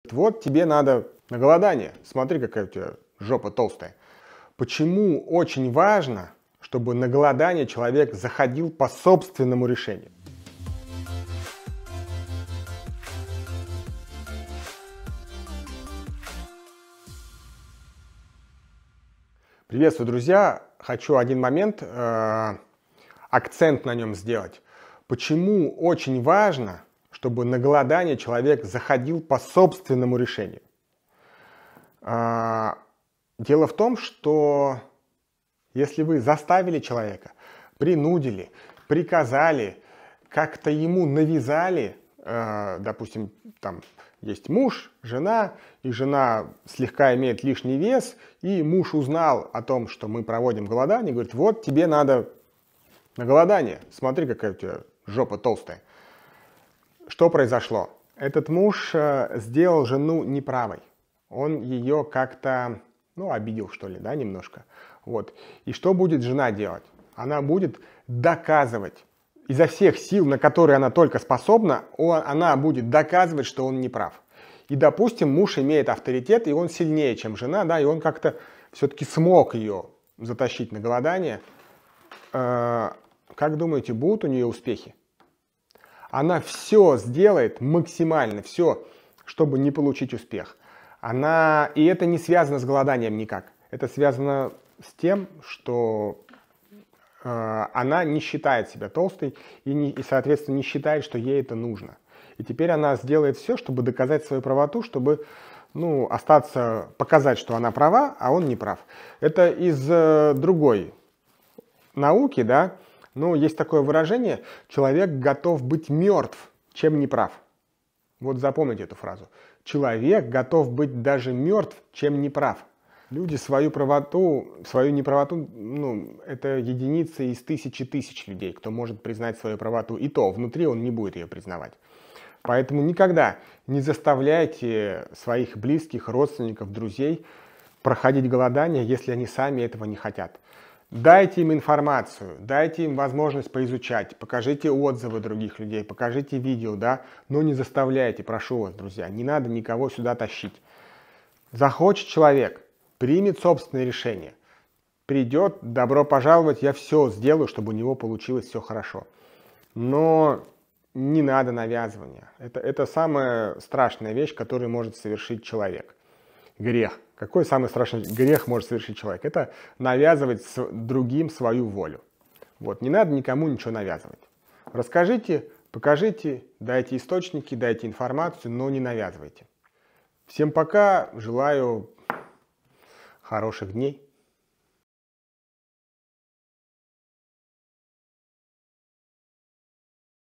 Почему важно не навязывать марафон голодания: уроки из опыта
Почему важно не заставлять и не навязывать человеку прохождение марафона по голоданию?
Из личного опыта я наблюдал многих людей, которые проходили марафон не по своему желанию. И у таких людей всегда либо маленькие результаты либо отрицательные результаты от марафона.

Человек должен сам хотеть, понимать и желать пойти на голодание, понимая для чего он это делает и с какой целью. Тех кому навязывают или заставляют цель одна, это доказать что голодание не работает.
Контакты для справок и вопросов:
Об авторе канала:
Смоляков Сергей Владимирович
автор книг "Рождённый болеть", "Научное голодание"